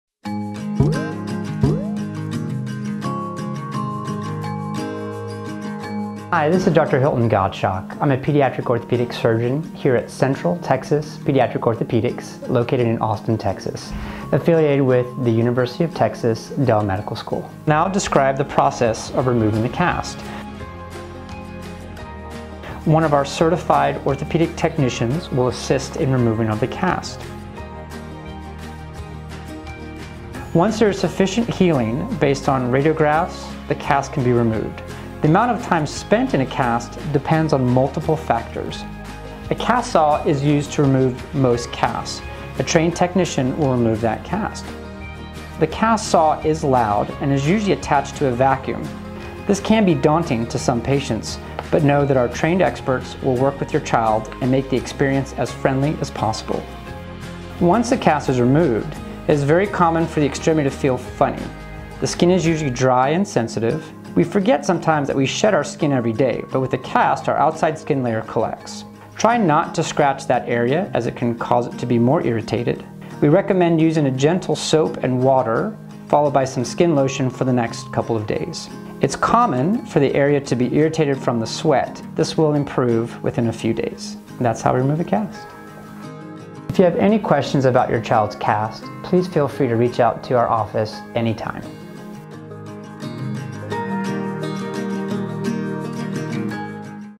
How are Casts Taken Off?
In this video, Dr. Hilton Gottschalk at Central Texas Pediatric Orthopedics discusses how doctors remove a plaster cast. If you're wondering how they remove a child's cast, join us for a brief overview.